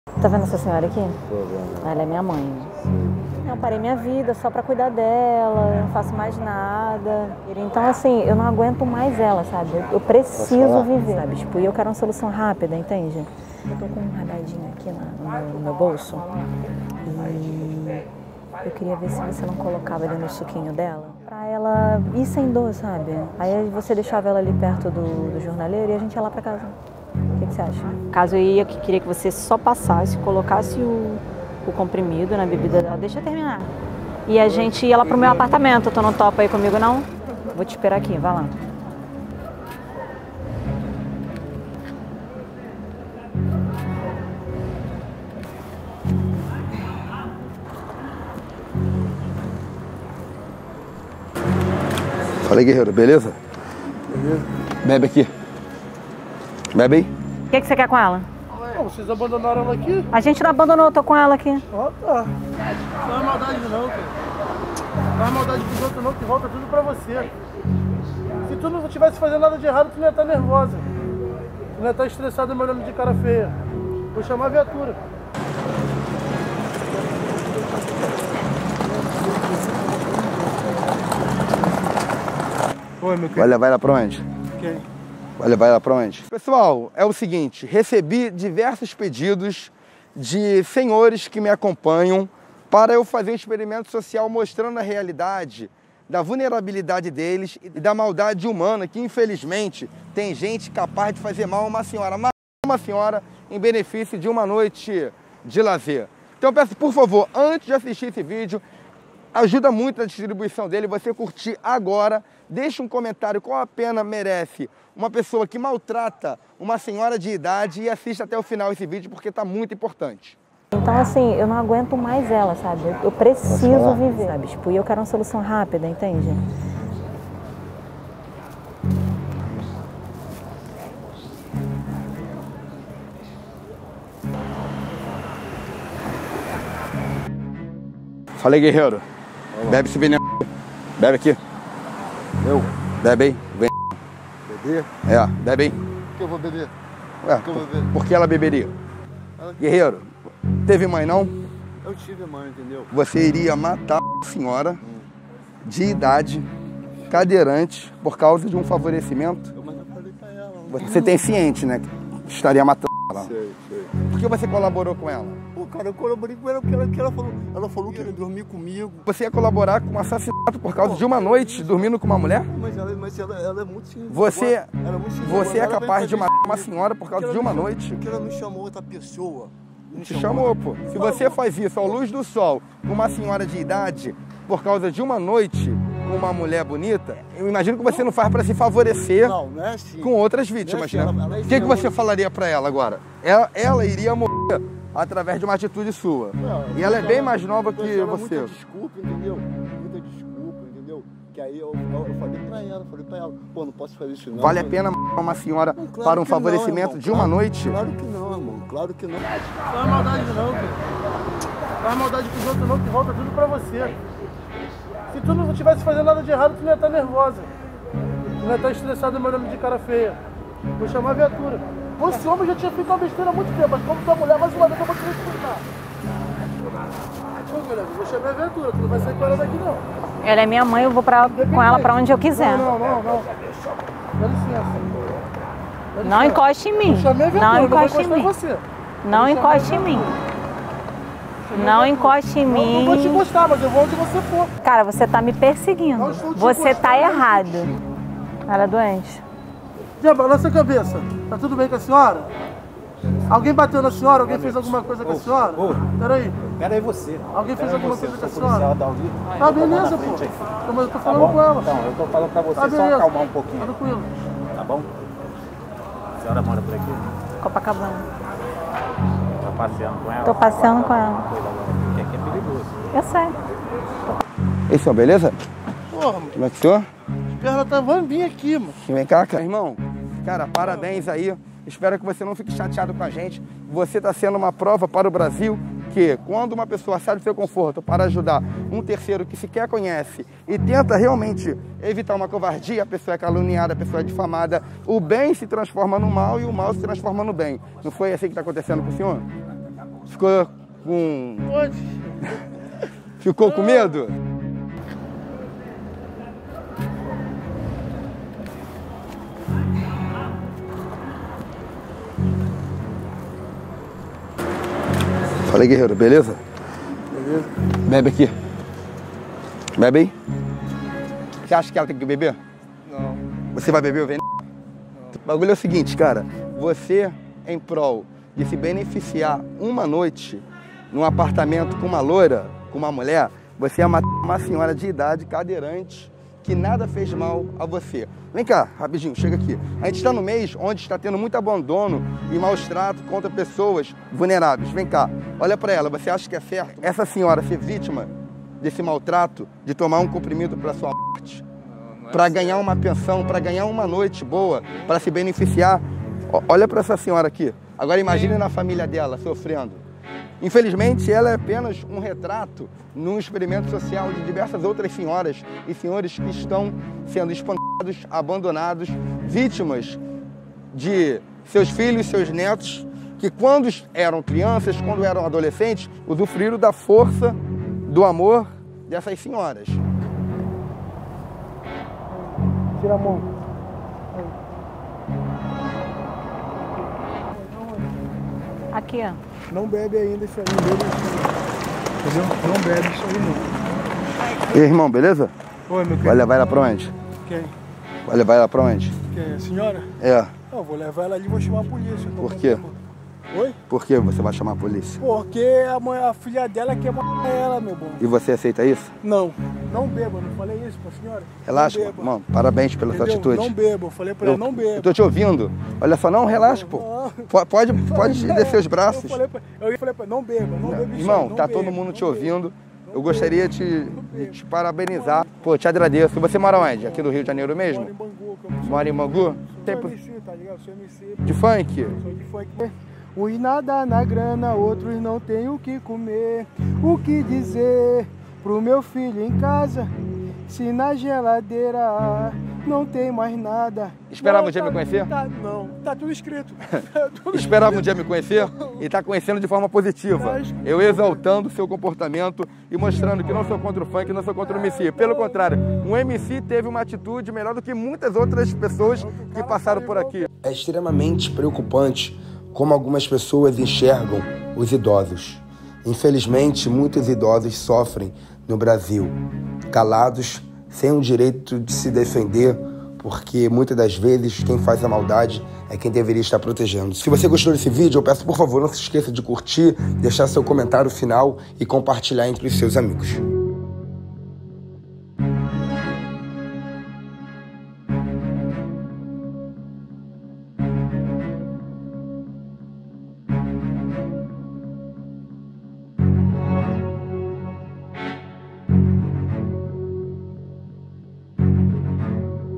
PEGAMOS EM FLAGRANTE HOMEM FAZENDO MAL A IDOSA, SE ARREPENDEU!
Até onde vai a maldade humana? Aceitar fazer mal a uma idosa cadeirante em troca de uma noite com a filha dela? Parece absurdo, mas tem gente capaz disso.

Neste experimento social, homens se dispuseram a ajudar nossa atriz a “SE LIVRAR“  da mãe, que cega e na cadeira de rodas não poderia se defender.

Assista este vídeo até o final, e deixe sua opinião.

Conteúdo exclusivo nas outras redes, preciso muito do seu apoio lá também.